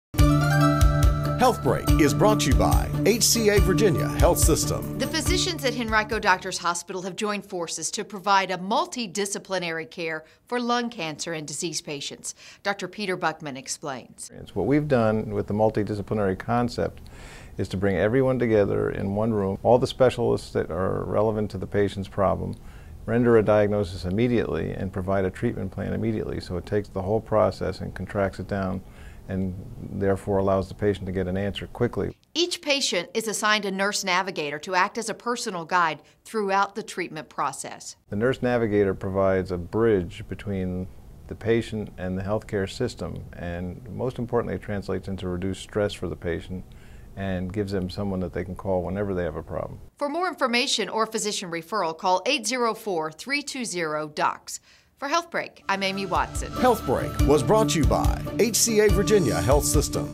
Lung Cancer is Treated with a Multi-Disciplinary Approach at Henrico Doctors' Hospital; Dr. Buckman
Lung Cancer is Treated with a Multi-Disciplinary Approach at Henrico Doctors' Hospital.  Dr. Peter Buckman explains the approach taken by the hospital and how it benefits patients suffering from lung cancer.